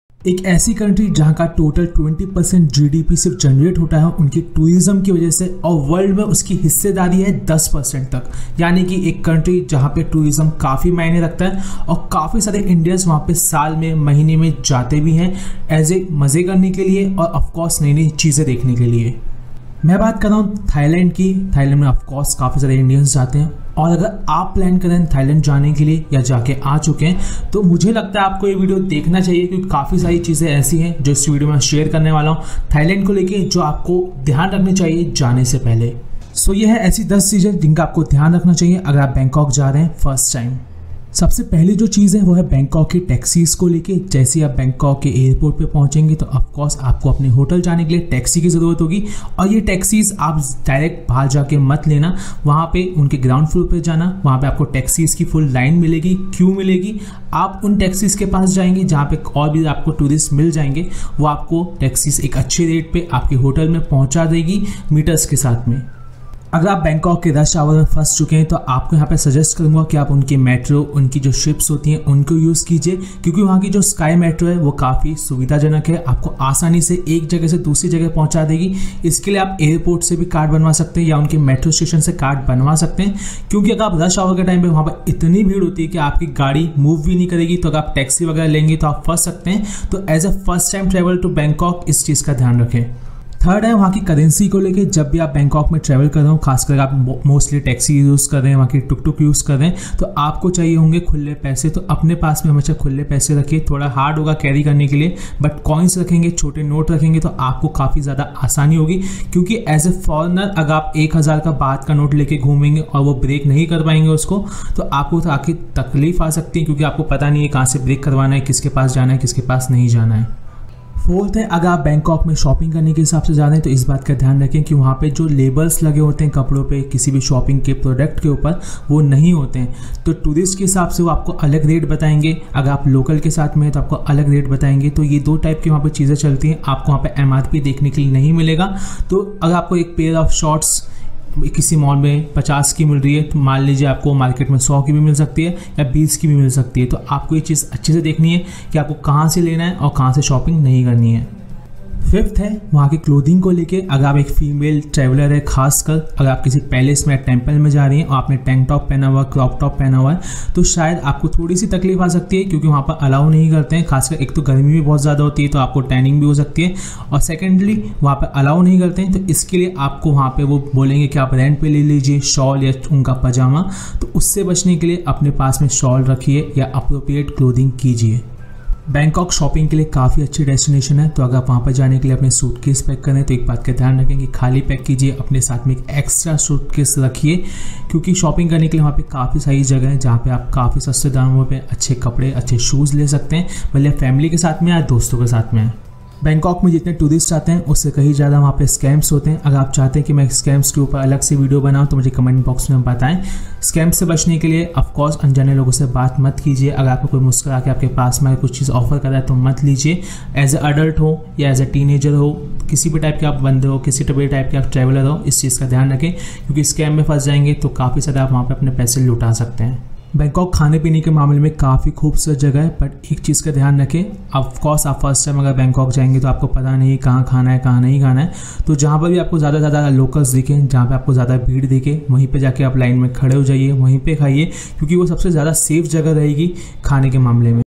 बैंकाक जाने से पहले जान लो ये बातें | Tips for First Time in Bangkok
If you are travelling to Bangkok for the first time I’m guessing you have already listened to enough tips, warnings and advice to write a book on the subject. Depending on who you talk to, Bangkok is portrayed as anything from a poverty stricken danger zone to a playground for dirty old men. Don’t listen to these, frankly, hysterical opitions.